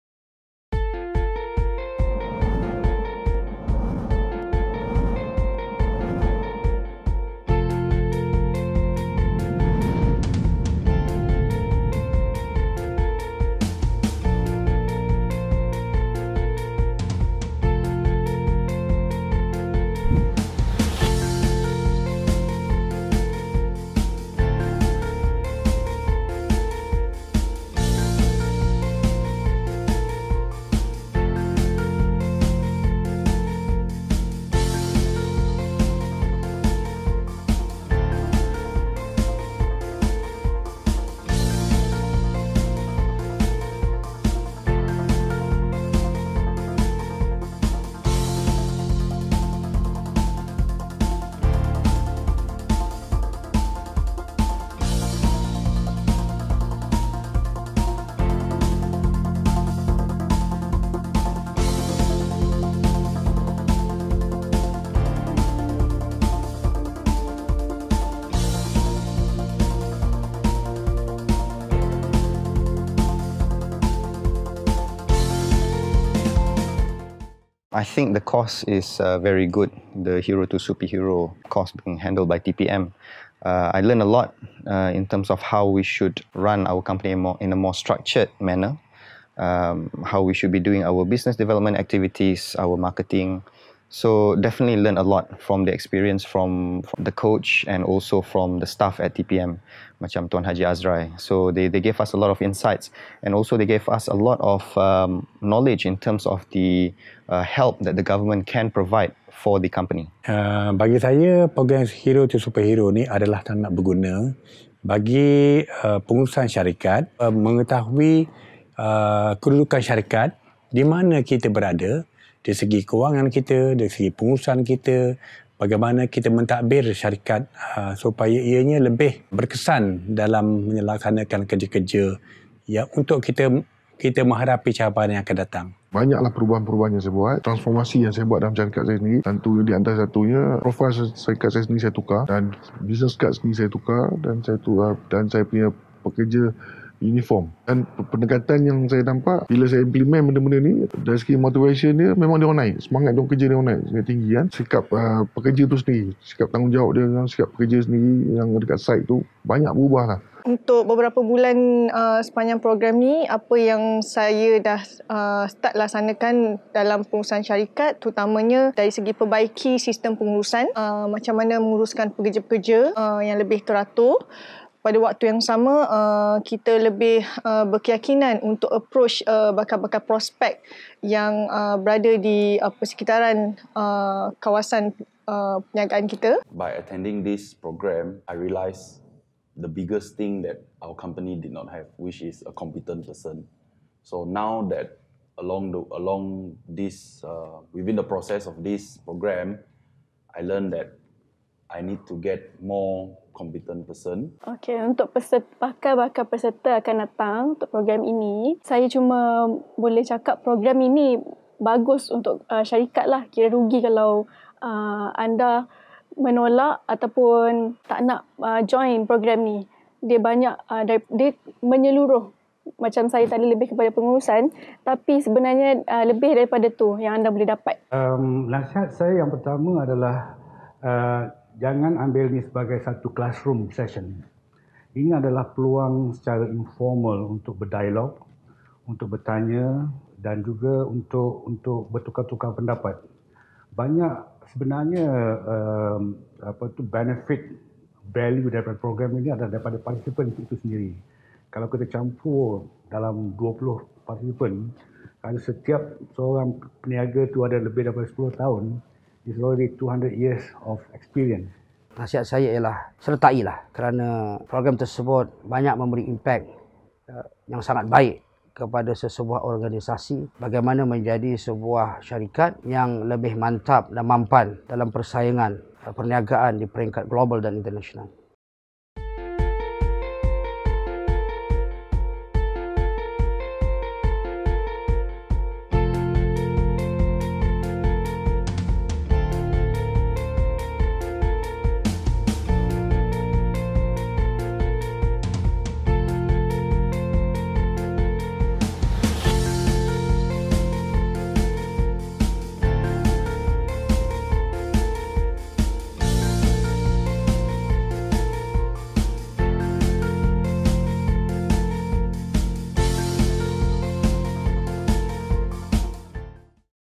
Hero To SuperHero Coaching Program
Testimonial Video from Participants of the Hero to Superhero Program 2018.